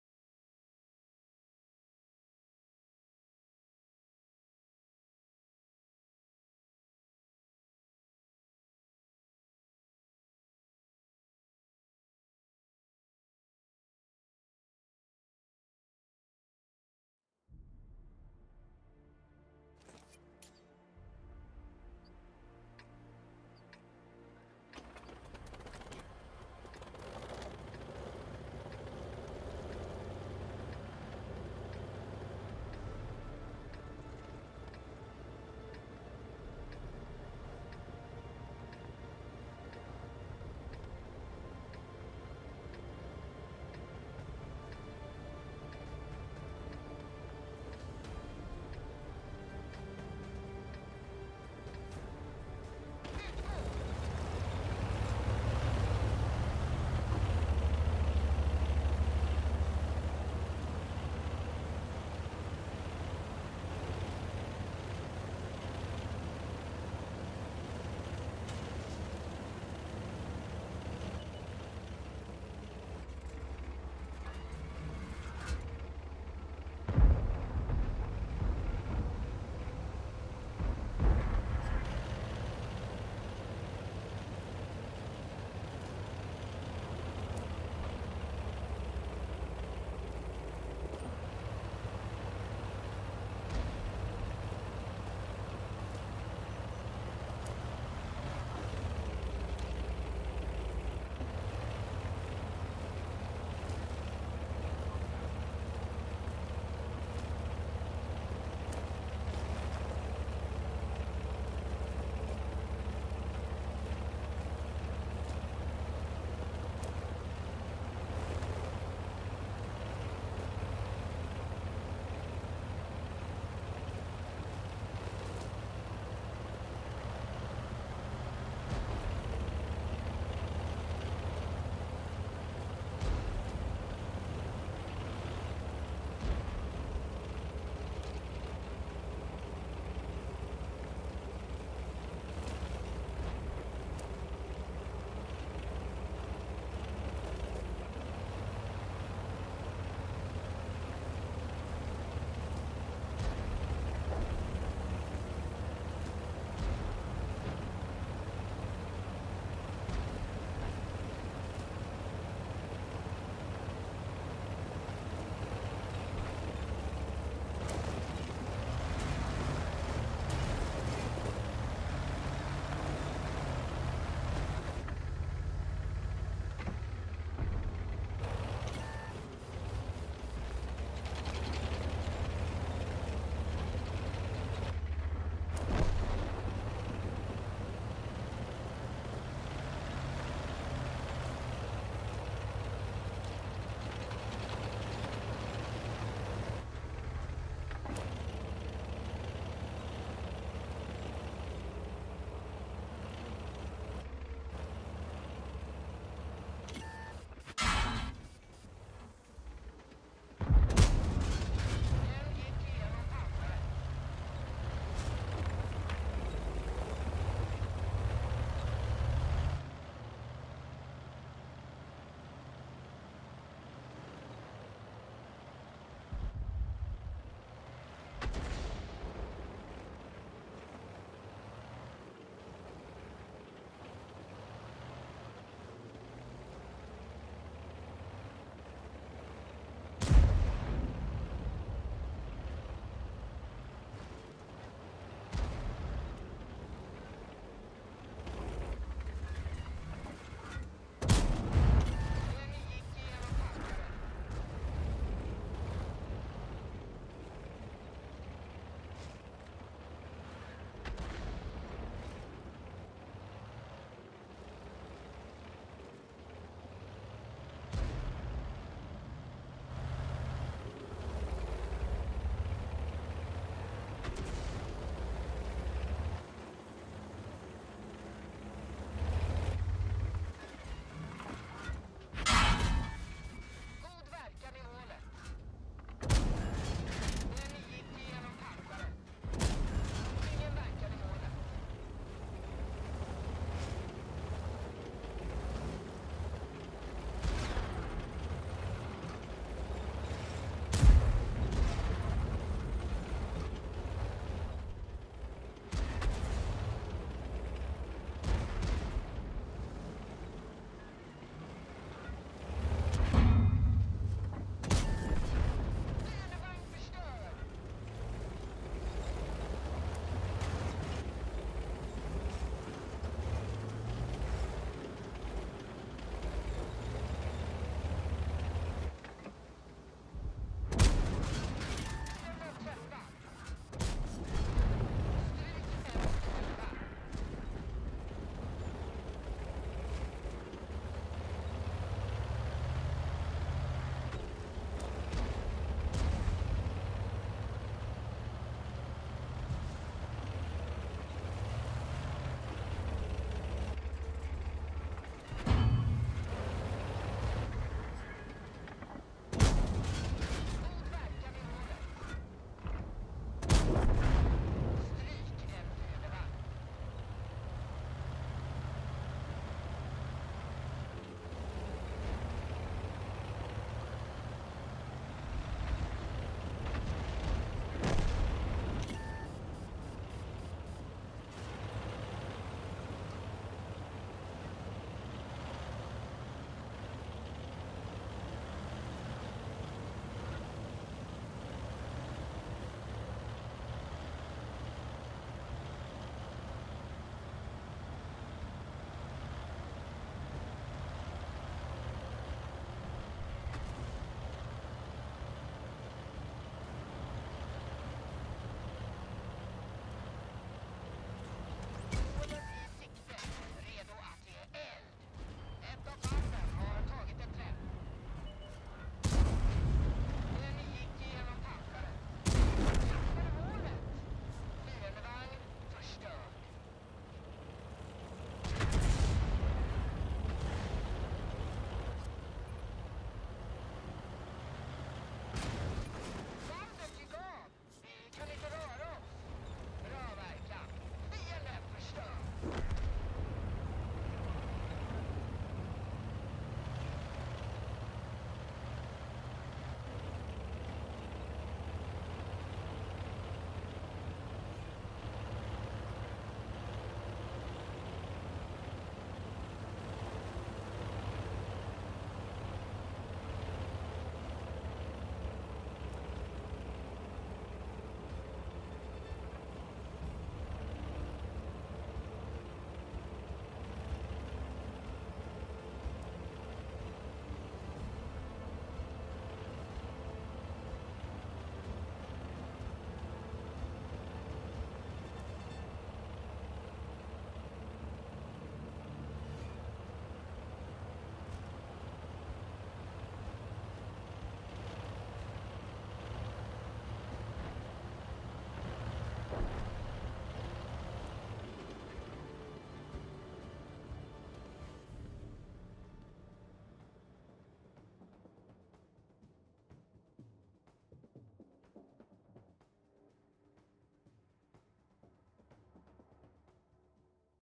Emill II - Malinovka - 1st - 5000 HP (HC), 5 Kills, 1000 spot
Tricky one this, wanted the hill but didn't get there in time even with a turbo
However, outplayed their Emill II and then the rest was easy